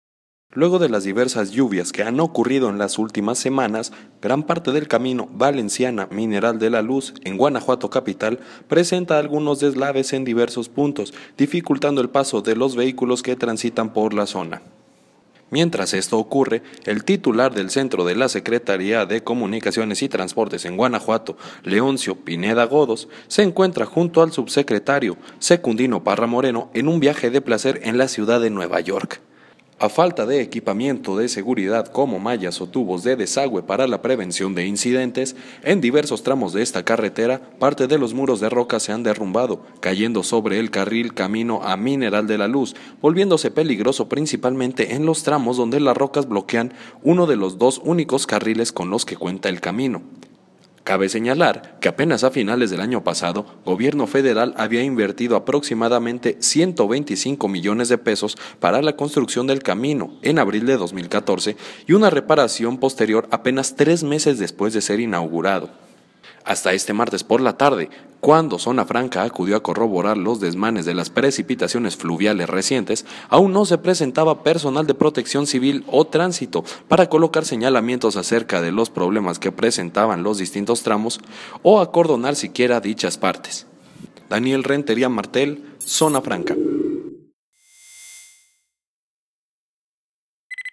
De nuevo, deslaves en camino Valenciana – La Luz; y el delegado de la SCT… en Nueva York
Luego de las diversas lluvias que han ocurrido en las últimas semanas,
gran parte del camino Valenciana – Mineral de la Luz, en Guanajuato
capital, presenta algunos deslaves en diversos puntos, dificultando el
paso de los vehículos que transitan por esa zona.